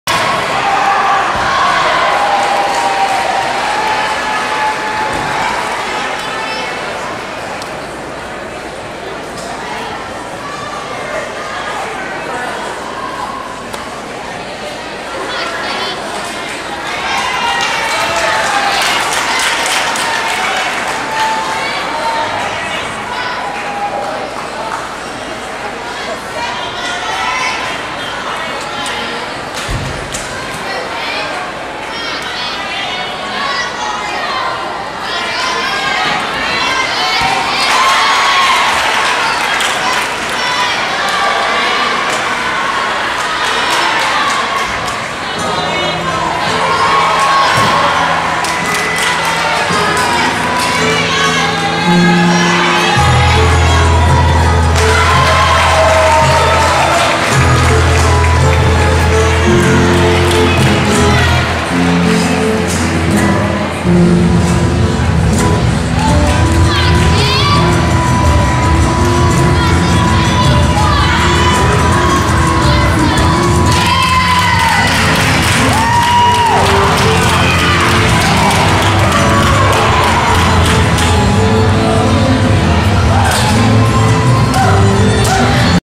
Kennedi Edney - Balance Beam - 2016 Women's Junior Olympic Championships
Score: 9.525
Senior D - May 8, 2016 - Fort Worth Convention Center - Fort Worth, Texas